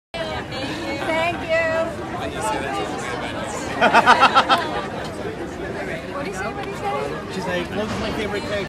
Tom Hiddleston at the stage door of Jacobs Theatre on September 30 2019
Tom Hiddleston at the stage door of Bernard B Jacobs Theatre after performing Betrayal on September 30, 2019.